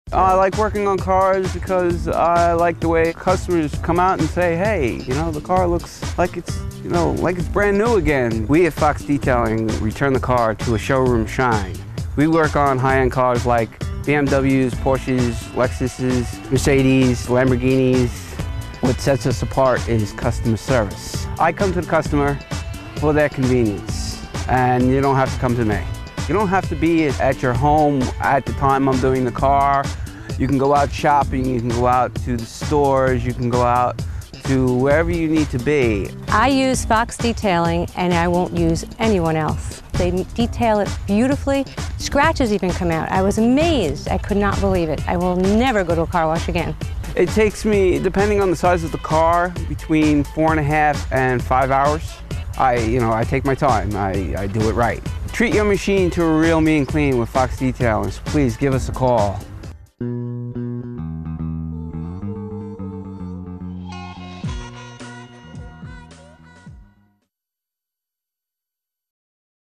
Fox Detailing
Joe Fox, owner of Fox Detailing in Centereach, New York (Long Island), wants your vehicle to shine and you to relax.  Specializing in high-end cars, Fox Detailing comes to your home or business and gets your car thoroughly clean.  Washing, interior cleaning, buffing, waxing, special attention to problem areas.  Call Fox Detailing and treat your machine to a real mean clean!